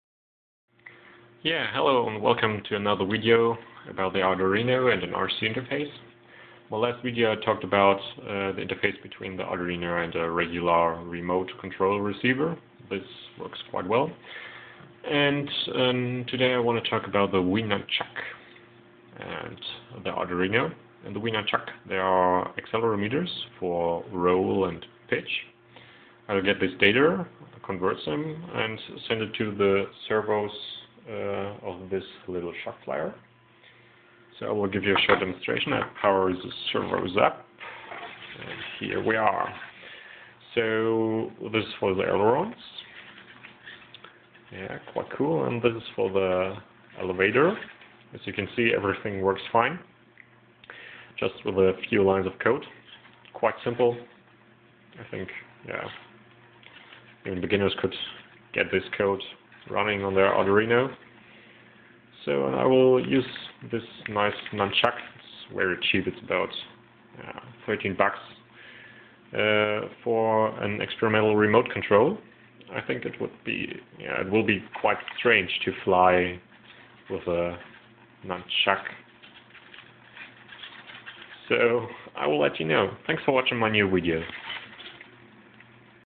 Wii Nunchuk + Arduino + Servo control
Here you can see my arduino interfacing with a Wii Nunchuk. The arduino reads the data from the acceleration sersors for the Nunchuk and converts them into a servo output. So i´m able to control the control surfaces of my littel plane. I think this will be a nice experimental remote Control.